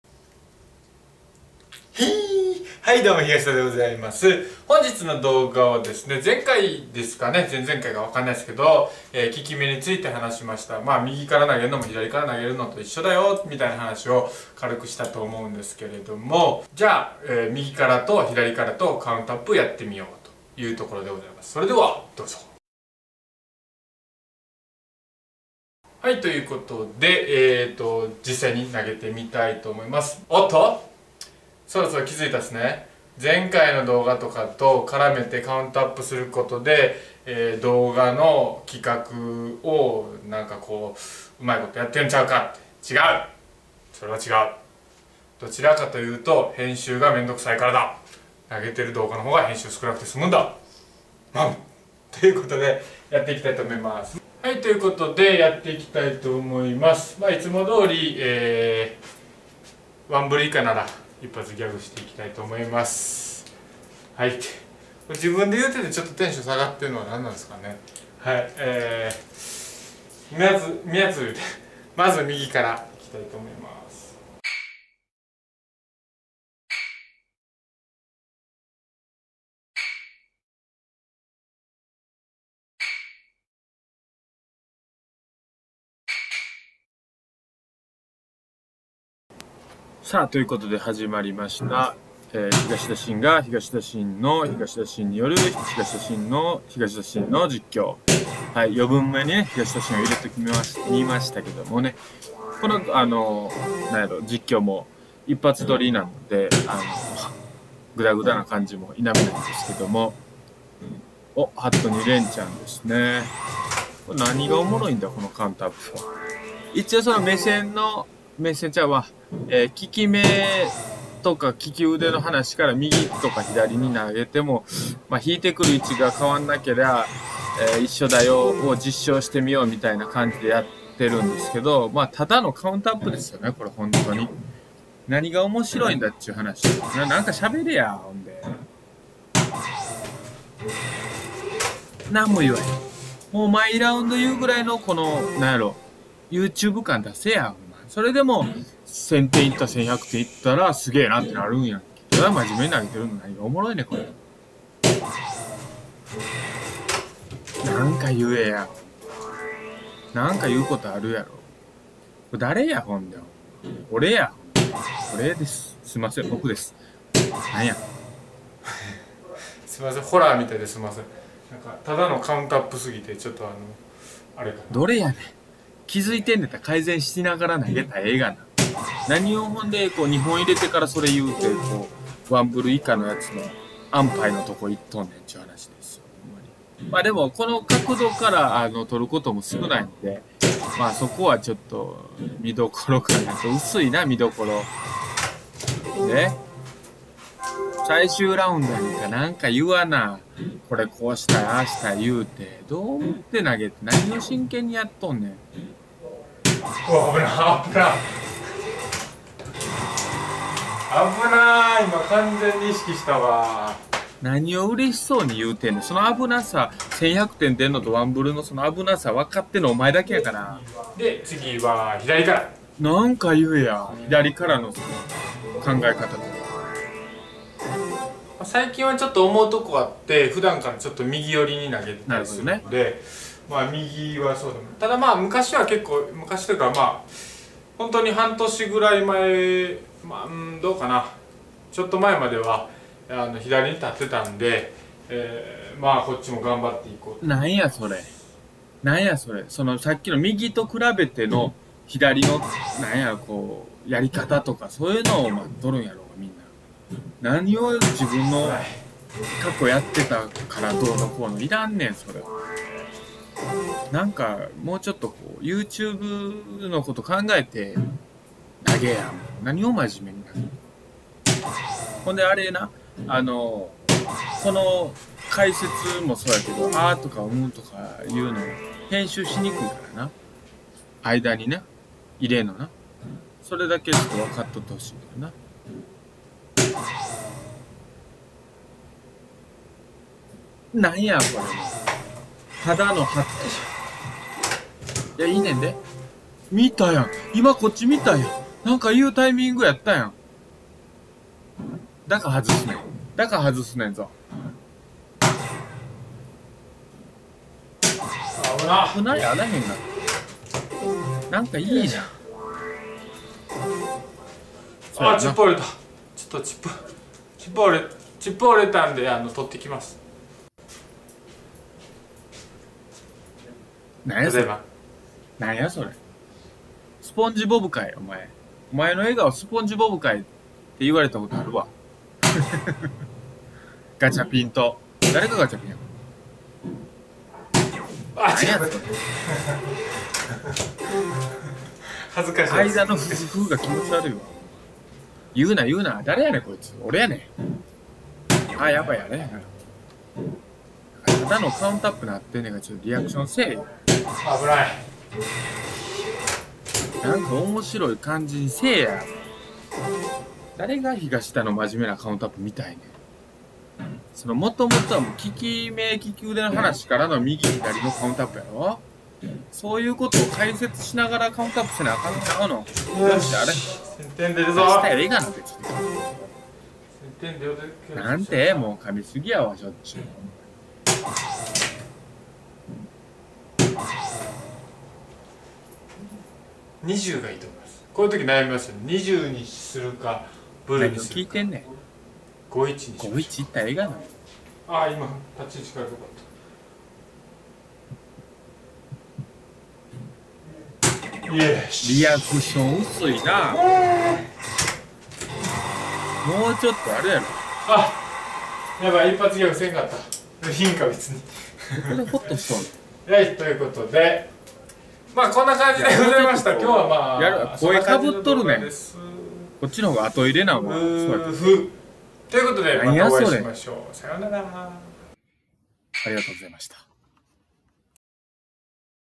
【ダーツ/左右の立ち位置】右からと左から投げてみた
今回の動画は前回の効き目の延長で左右の立ち位置で投げて
実際どうかという感じだったんですが、ただのカウントアップ
動画になってしまいました
カメラ位置とかあまりない角度なので、何らかの参考になればと・・・

暖かい目で見ていただけると幸いです☆

【ダーツプロ育成企画】
出演中の西川 綾ちゃんの成長日記

☆東田が使っているダーツ

★東田のフライト・シャフト